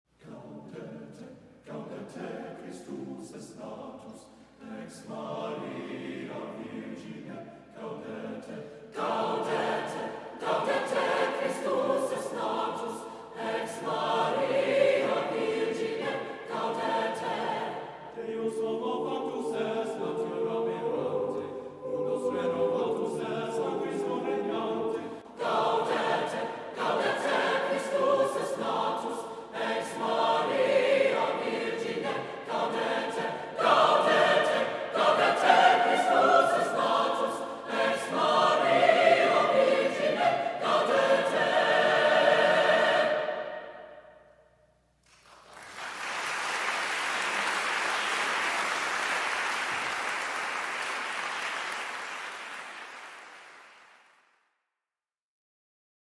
Birmingham Voyces 2011
A brief trailer introducing the latest choral endeavor by conductor/composer/music educator Timothy Paul Banks ... to be launched in 2011 ... a combination of virtual and actual choir ... performing a wide and varied repertoire representing the choral art throughout its 500-year history ... stay tuned .